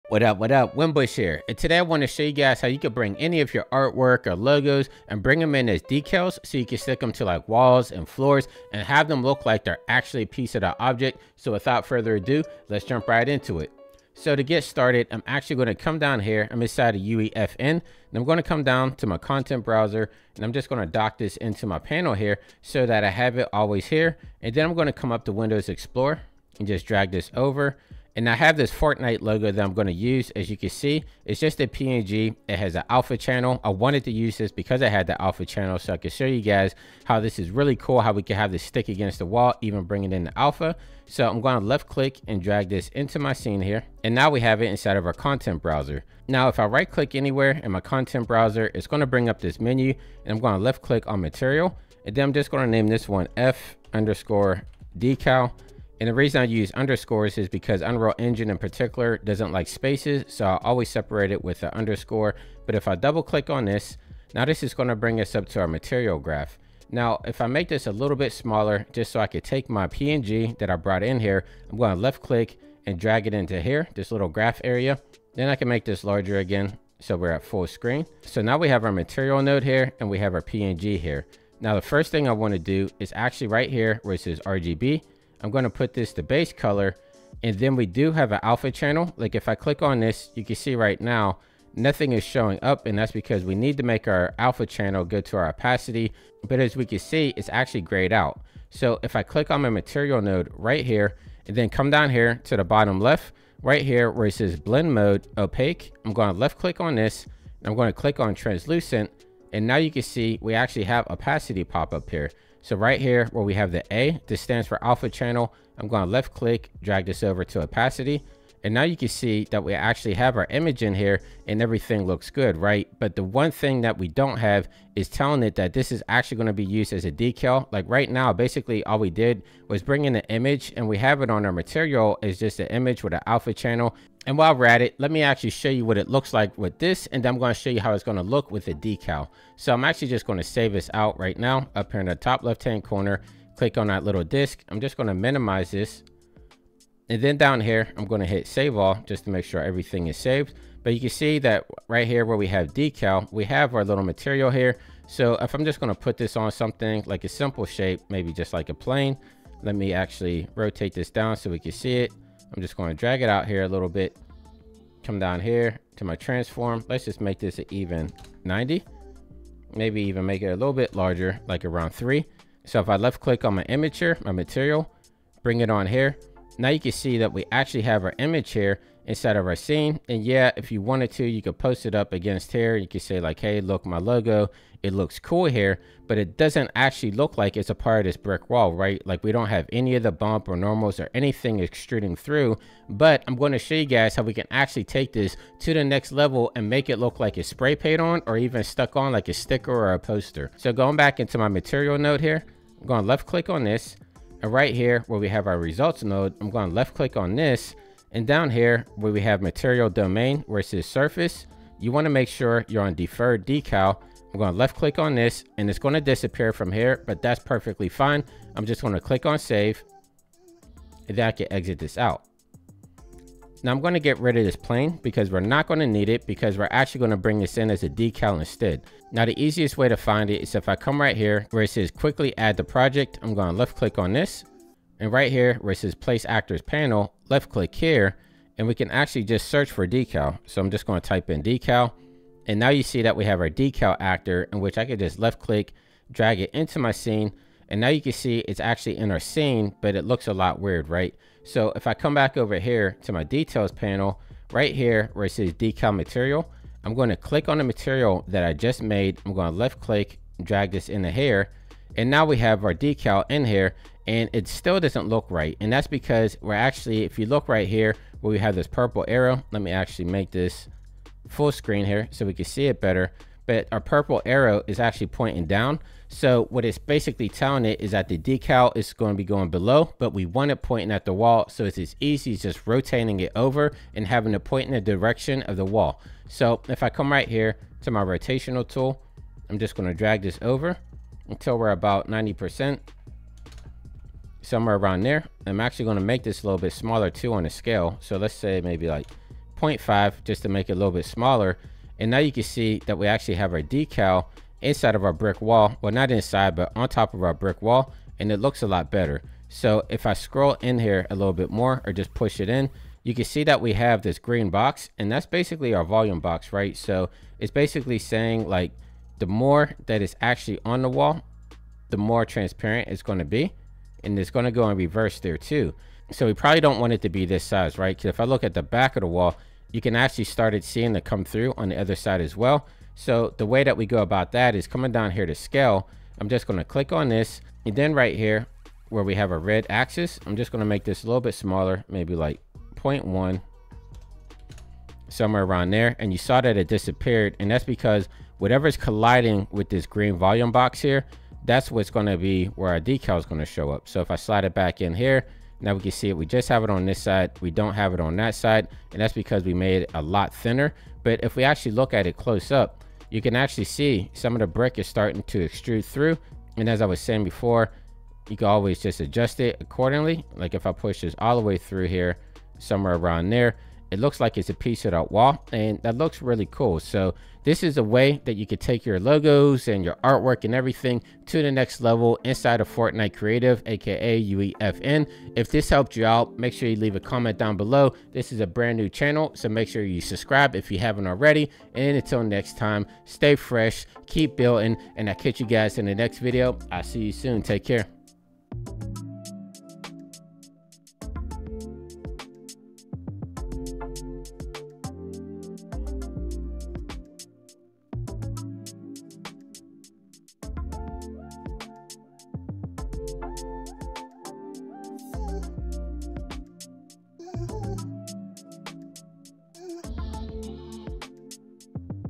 Create Decals for Unreal Engine 5 & UEFN like a Pro | Winbush Tips and Tricks
In this video, I will show you how to create awesome decals for Unreal Editor for Fortnite UEFN and or Unreal Engine 5. Decals are 2D textures that you can apply to surfaces on your island to add details, effects, or decorations. You will learn how to import decals from external sources, how to use the decal actor and the decal material, and how to adjust the properties and settings of your decals. I will also share some tips and tricks from my experience as a professional artist on how to use decals effectively and creatively in your UEFN projects. By the end of this video, you will be able to create stunning artwork in UEFN using decals.

My New School Of Motion Course "Unreal Engine  for 3D Artists"

I partnered with Ekster to give you an exclusive discount.

0:00 What up what up
0:14 Getting Started
1:18 Connecting Nodes
2:27 Art with Alpha Example
3:20 Decal Setup